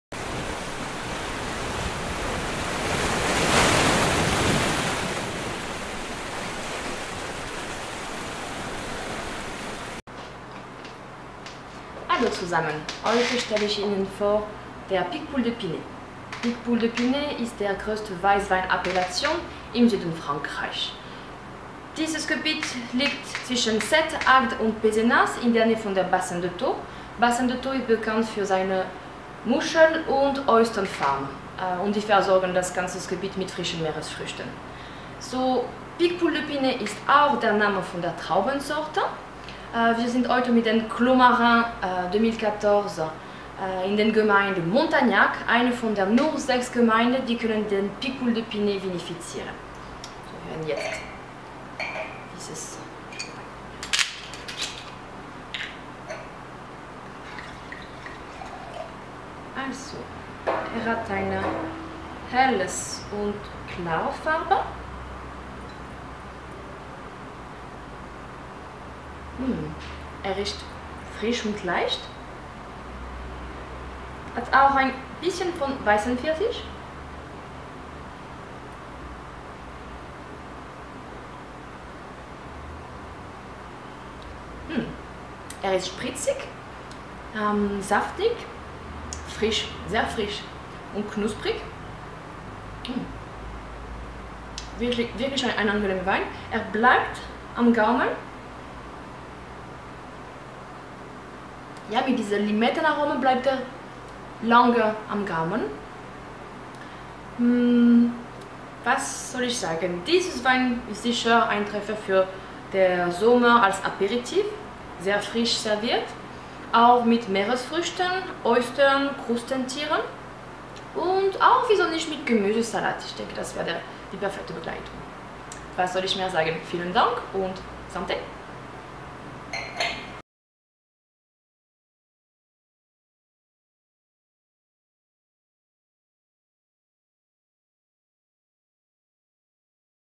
Flo's Weinpräsentation Picpoul de Pinet Clomarin
Flo stellt Ihnen den Wein Bella Dama von Château Saint Lucie d'Aussou aus der Region Corbières Boutenac vor.